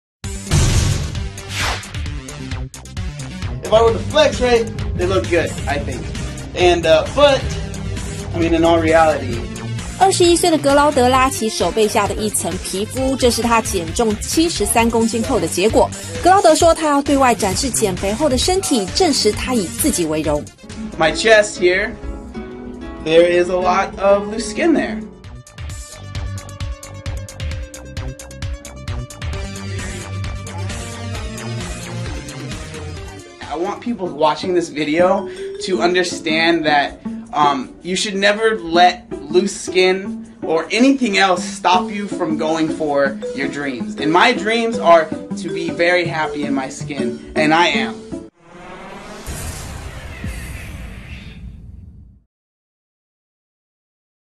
減肥男自白--蘋果日報 20141112
【動新聞╱綜合報導】美國一名21歲男子原本體重高達163公斤，但他靠著健康飲食以及運動，成功瘦下73公斤。為了鼓勵自己及他人，他特地拍攝一段影片，勇敢秀出減肥後的身體贅皮。

格勞德（John David Glaude）在鏡頭前先展示他手臂的肌肉，接著拉起手臂下的贅皮。然後格勞德脫下衣服，用力拉他胸部以及肚子上的贅皮。格勞德說，原本他對自己脫下衣服的模樣沒有什麼信心，但他後來想開了，這就是他過去減肥成功的證明。格勞德在影片中說，未來他將繼續與這些皮膚快樂共處，因為這就是他能走到這一步的象徵，而他也會繼續減肥直至達到理想目標為止。